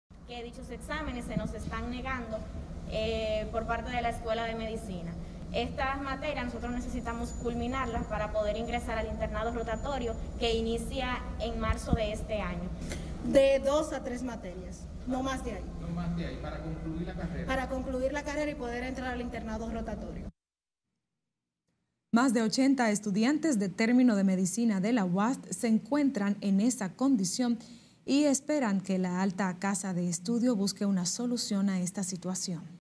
Estudiantes de medicina de la UASD protestan
Estudiantes de Medicina de la Universidad Autónoma de Santo Domingo (UASD) exigieron hoy a las autoridades de la universidad estatal programar las materias especiales que forman parte del currículo educativo, para ellos poder concluir su ciclo formativo.